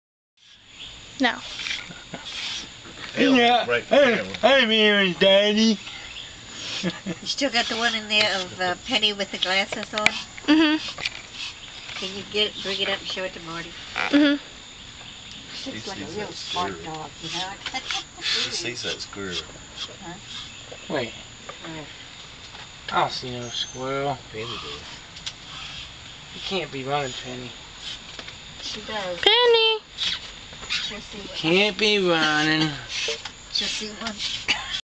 Daddy. 2008
One of the last videos I have of my dad.
David Wayne Railey.